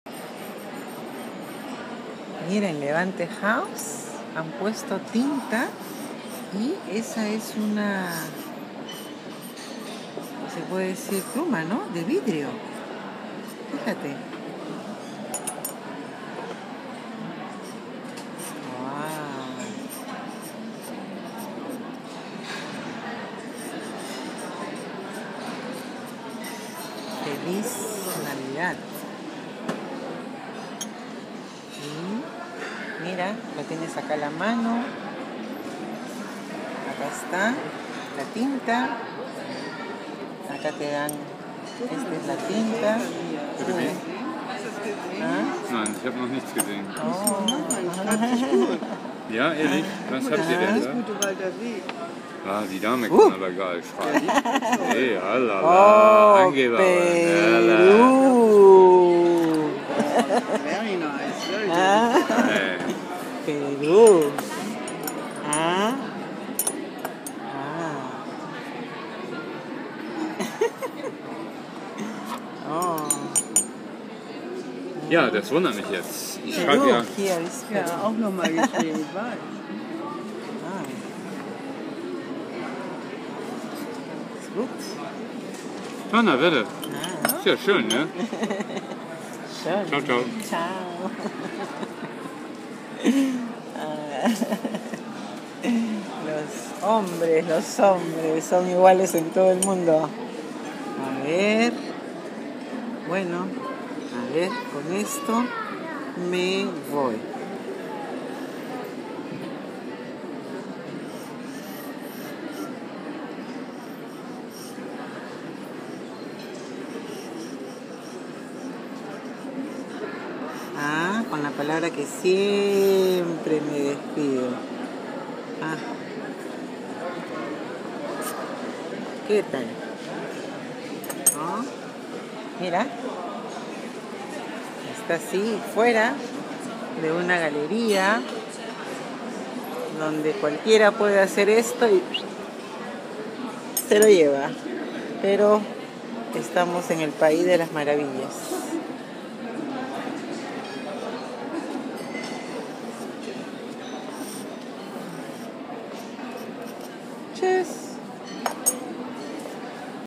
Tinta y plumas de vidrio en una galería GRATIS
La cultura Alemana es como la Japonesa, nadie roba, todo está a la mano y la gente usa todo y lo deja en su sitio, una maravilla